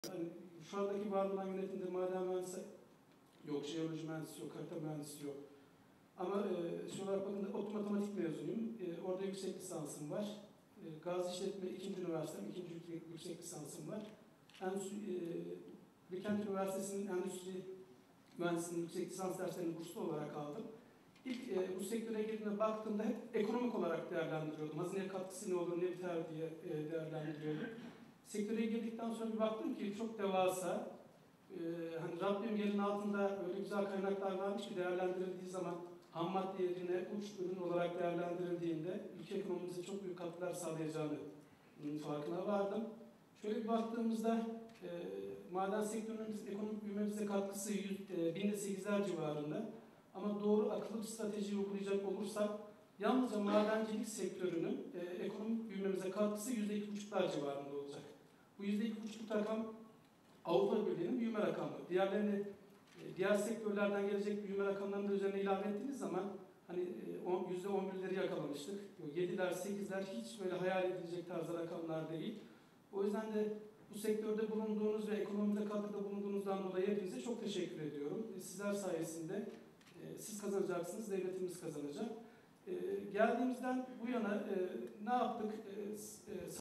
‘Siz kazanacaksınız, devletimiz kazanacak’
Enerji ve Tabii Kaynaklar Bakanlığı Maden İşleri Genel Müdürlüğü heyeti Afyonkarahisar Ticaret ve Sanayi Odası’nda Sektör Toplantısı gerçekleştirdi. Toplantıda konuşan Maden İşleri Genel Müdür Yardımcısı Vekili Uğur Salih Uçar, Afyonkarahisar’ın maden sektöründe önemli bir yere sahip olduğunu farkında olduklarını ifade ederek, sektör temsilcilerine gereken desteği vereceklerini söyledi.